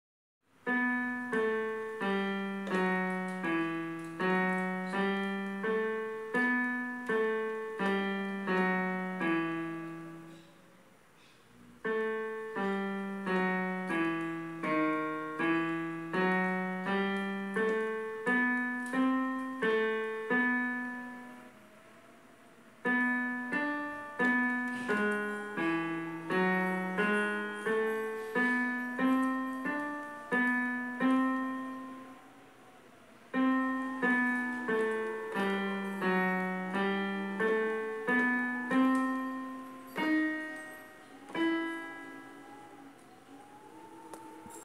Leitura Métrica - 224 - Curso Completo de Teoria Musical e Solfejo, vol. 1
Pianista : Matheus Santos
Por : Belmira Cardoso e Mário Mascarenhas
Editora : Irmãos Vitale
Edição : 14a